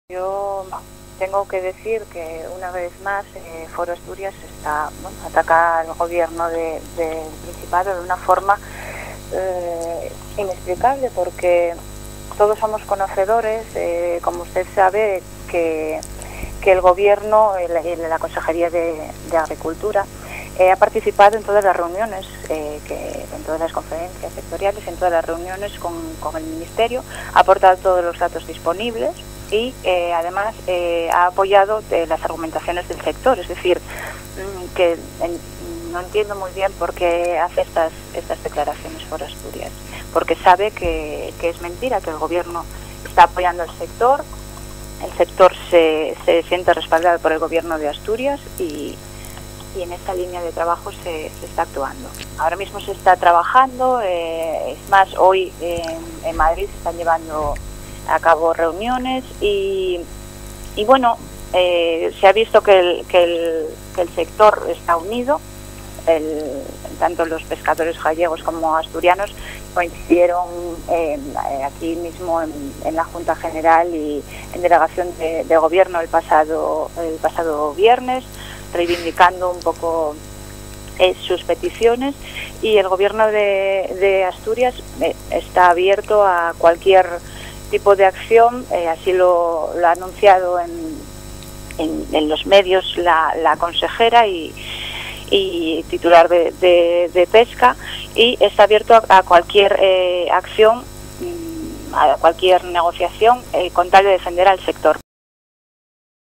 ORT PSOE Reparto Cuota Pesca 17-03-2014
La diputada socialista Elsa Pérez, portavoz de Pesca en el parlamento regional, recuerda que la Consejería de Agroganadería ha mostrado siempre su respaldo al sector pesquero. Con respecto a la búsqueda de un frente común, señala que el gobierno regional está abierto a cualquier defensa del sector, y considera las acusaciones de Foro como un nuevo ataque al gobierno regional.